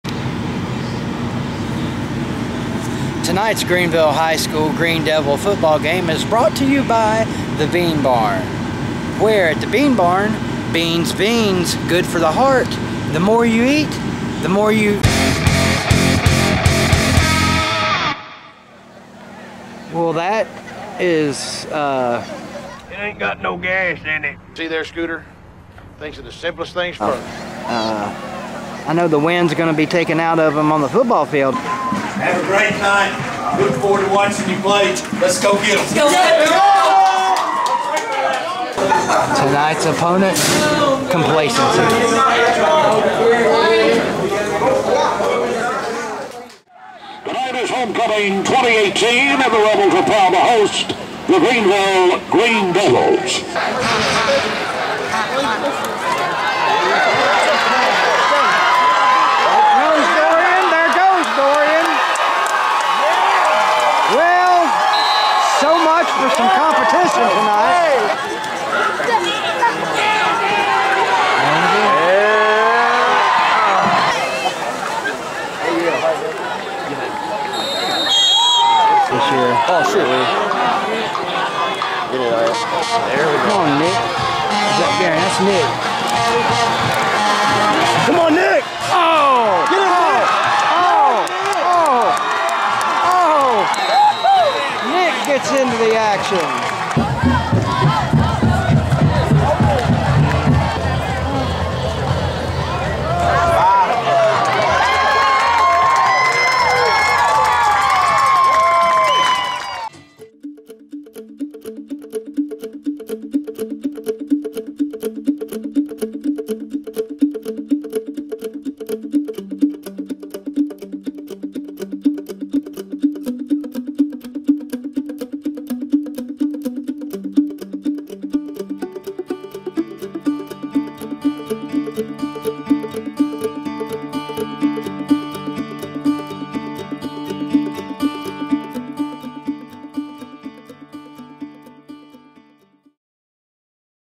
Devils finally get some competition
7-0 after pounding Sullivan south, the Devils now "welcome" ole bitter rival Betsy to town this week